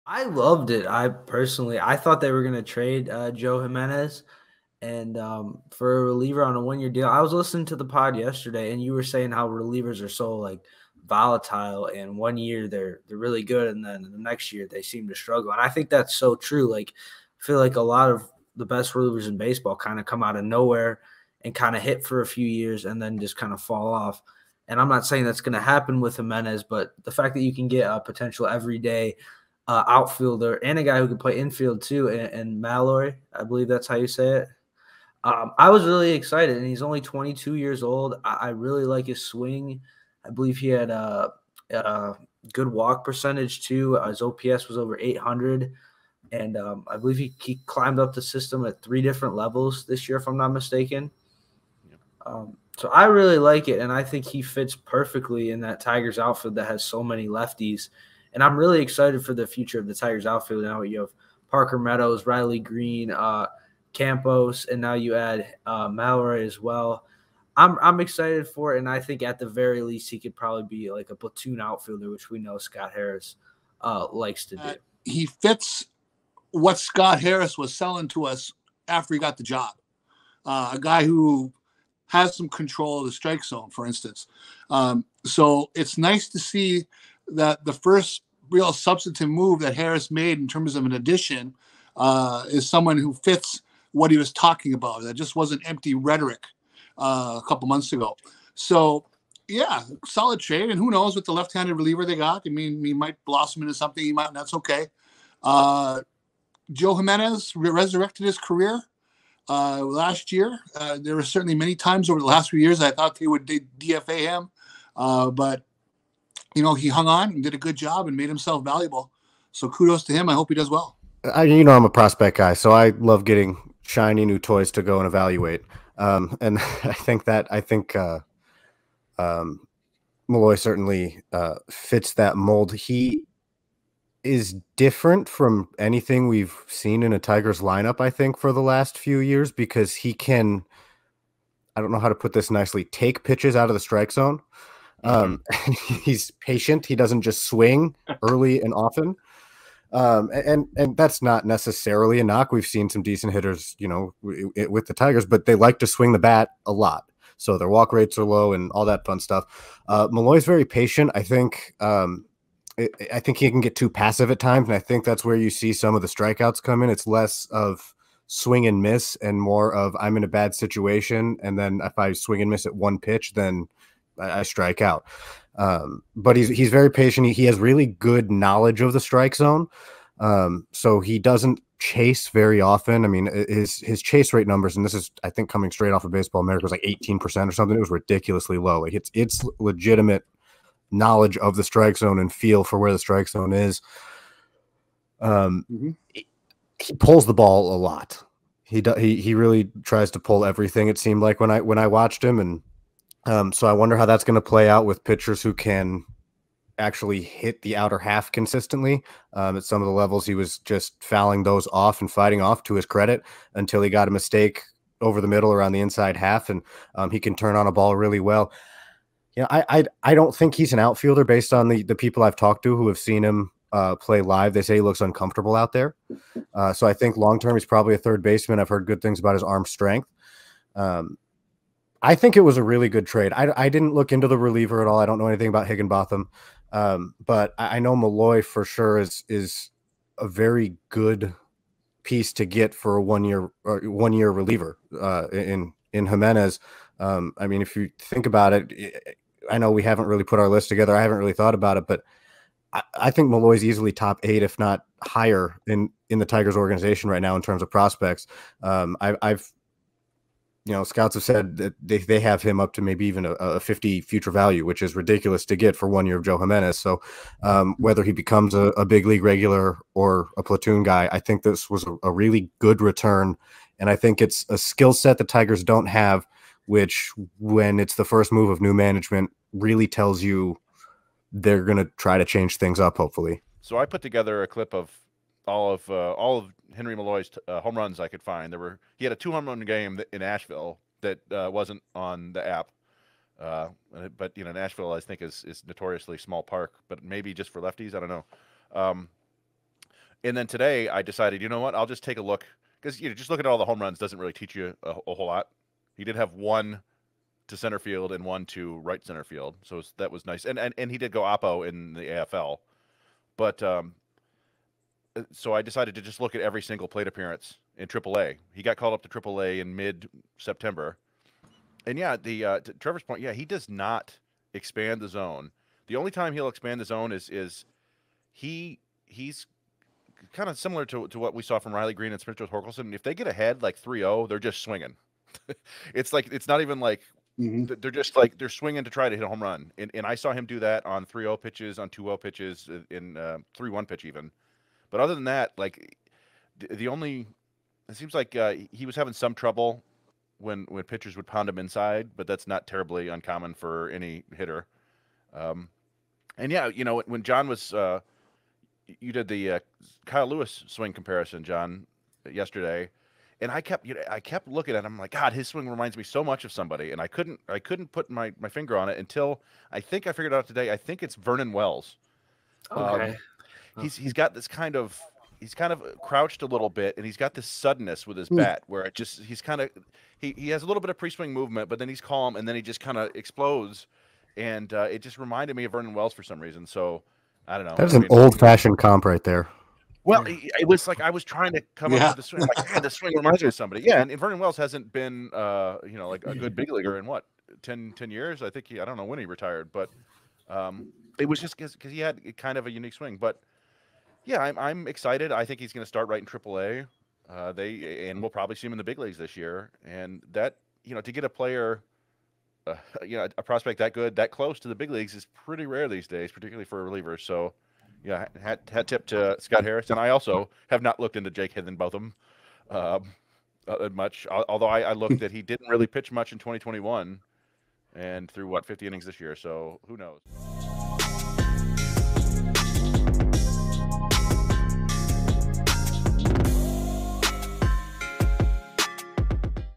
What Does New Prospect Justyn-Henry-Malloy bring to The Detroit Tigers?
Chris, Jon, Yooper and Trevor talk about newly acquired prospect Justyn-Henry Malloy and what he be in the future for the Tigers.